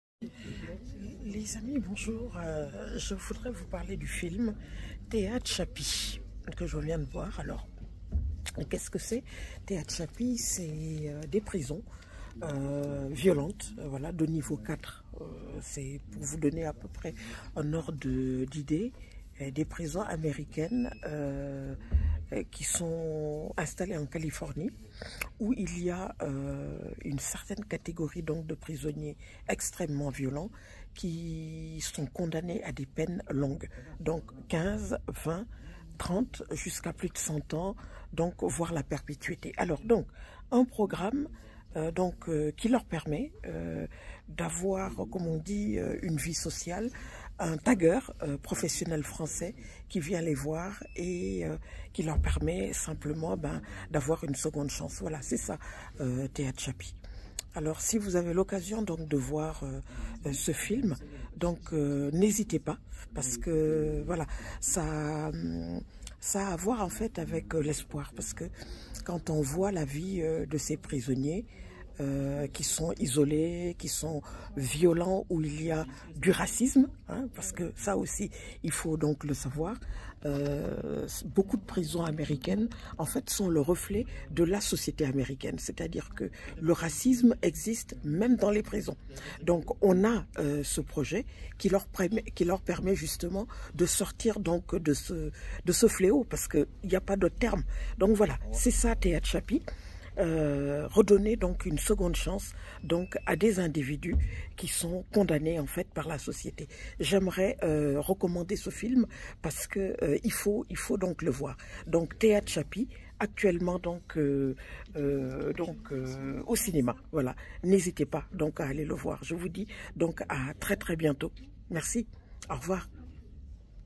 Film TEHACHAPI de JR OU COMMENT REDONNER ESPOIR À DES PRISONNIERS DE L'ENFER CARCEEAL AMERICAUN
Film
TEHACHAPI DE JR
 OU COMMENT REDONNER ESPOIR À DES PRISONNIERS DE L'ENFER CARCEEAL AMERICAUN
JE RECOMMANDE LE FILM